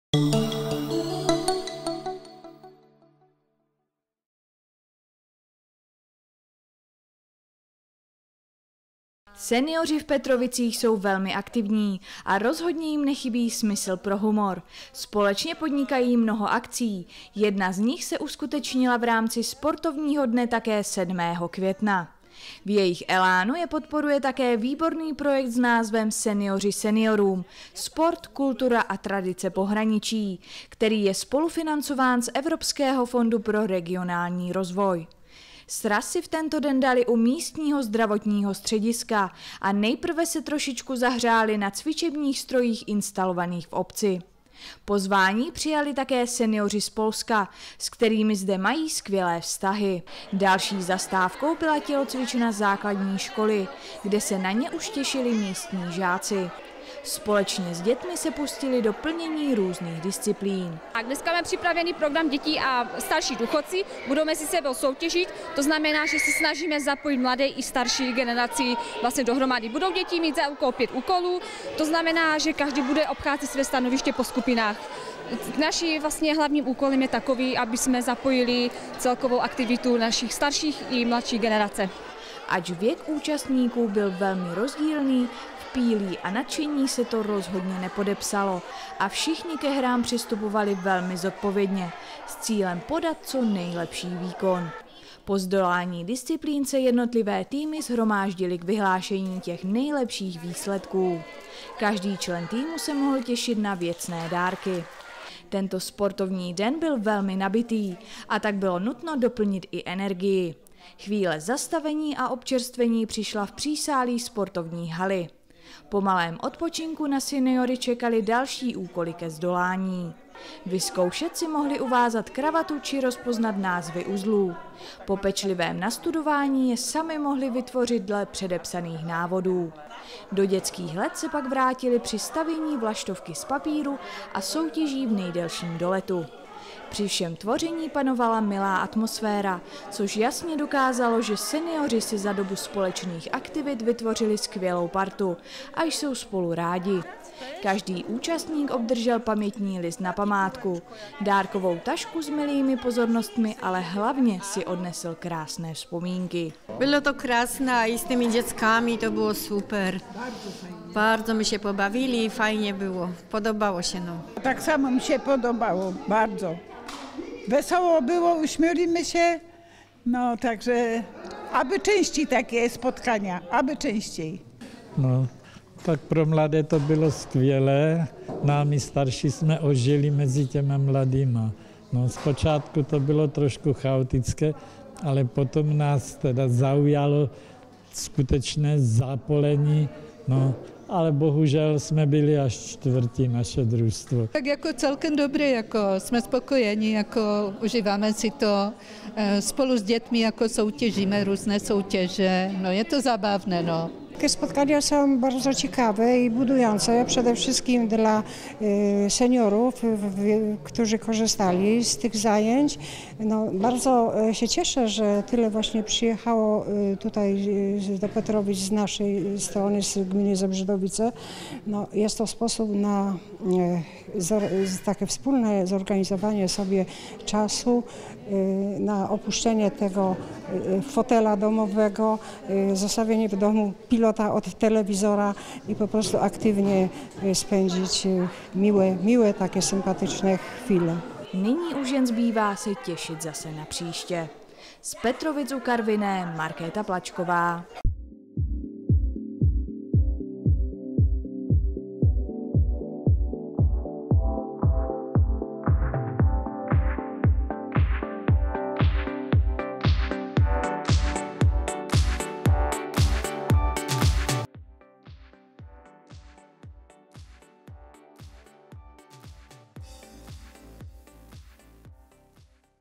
Petrovice u Karviné - Projekt Senioři seniorům - Sportovní den
Aktualizované: Senioři v Petrovicích jsou velmi aktivní a rozhodně jim nechybí smysl pro humor, společně podnikají mnoho aktivit, jedna z nich se uskutečnila v rámci sportovního dne také 7. Května. V jejich elánu je podporuje také výborný projekt s názvem "Senioři seniorům - sport, kultura a tradice pohraničí", který je spolufinancován z Evropského fondu pro regionální rozvoj. Sraz si v tento den dali u místního zdravotního střediska a nejprve se trošičku zahřáli na cvičebních strojích instalovaných v obci. Pozvání přijali také senioři z Polska, s kterými zde mají skvělé vztahy. Další zastávkou byla tělocvična základní školy, kde se na ně už těšili místní žáci. Společně s dětmi se pustili do plnění různých disciplín. Ač věk účastníků byl velmi rozdílný, v píli a nadšení se to rozhodně nepodepsalo a všichni ke hrám přistupovali velmi zodpovědně s cílem podat, co nejlepší výkon. Po zdolání disciplín se jednotlivé týmy shromáždily k vyhlášení těch nejlepších výsledků. Každý člen týmu se mohl těšit na věcné dárky. Tento sportovní den byl velmi nabitý a tak bylo nutno doplnit i energii. Chvíle zastavení a občerstvení přišla v přísálí sportovní haly. Po malém odpočinku na seniory čekaly další úkoly ke zdolání. Vyzkoušet si mohli uvázat kravatu či rozpoznat názvy uzlů. Po pečlivém nastudování je sami mohli vytvořit dle předepsaných návodů. Do dětských let se pak vrátili při stavění vlaštovky z papíru a soutěží v nejdelším doletu. Při všem tvoření panovala milá atmosféra, což jasně dokázalo, že senioři si za dobu společných aktivit vytvořili skvělou partu a jsou spolu rádi. Každý účastník obdržel pamětní list na památku, dárkovou tašku s milými pozornostmi, ale hlavně si odnesl krásné vzpomínky. (anketa) Nyní už jen zbývá se těšit zase na příště.